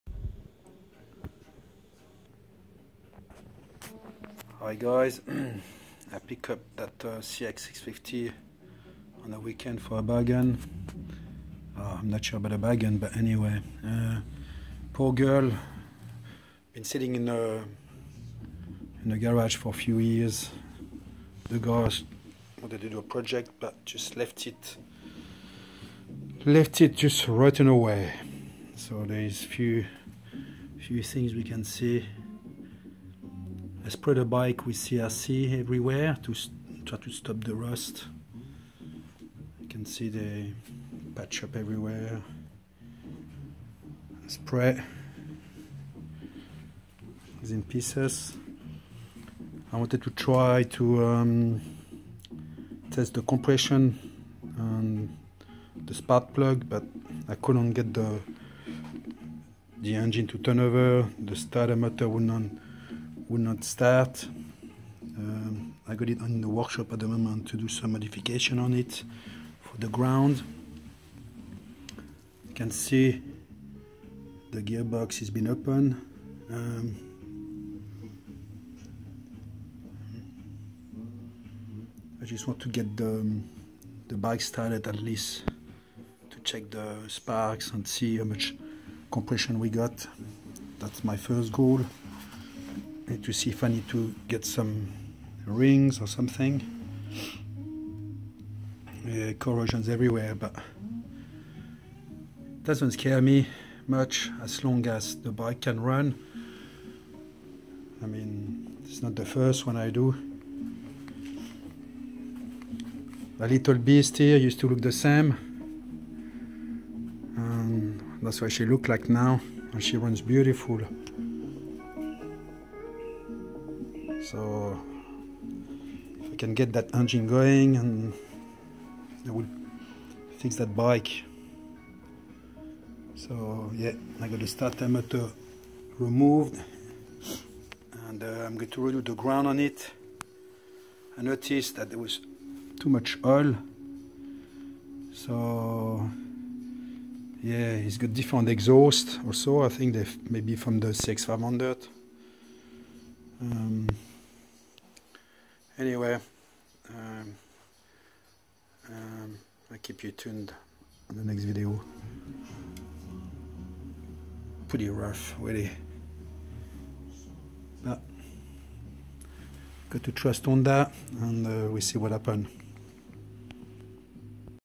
CX650 E - Cafe Racer - "RUSTY" - as found first look
Found this CX650 E in pieces, would not turn over, was left to rust outside for many years. Later on I found out the issues, one of them was the common  starter clutch.
After viewing a video of a GL650 that was turned into a Cafe Racer, when I heard the sound of it I fall in love with it and started the search for one!
I was lucky enough to found one still registered, all the pieces was there but was a bucket of rust.
That's when I decided to keep the rust, create even more and build a rough and rusty Cafe Racer. I named her "Rusty", She has the bones from at least 5 different motorcycles
The CX is a beautiful bike but painful process when something goes wrong you have to remove the whole motor.
This is my 1st Honda and 1st CX650 E, later on I end up doing 2 more, one back to original, one into a Tracker. both got sold in the last 2 years
Rusty has over 100'000 kms and was restored 3 years ago, as this day she is still running and going well, same pistons and rings. She is my favorites of all the bikes I build so far. She is looked after, sleep under a blanket when not ridden.
Thank you for looking. My accent is French from Switzerland but I live in Australia, if you struggle to understand me, just ask me the question and will answer you.
Sam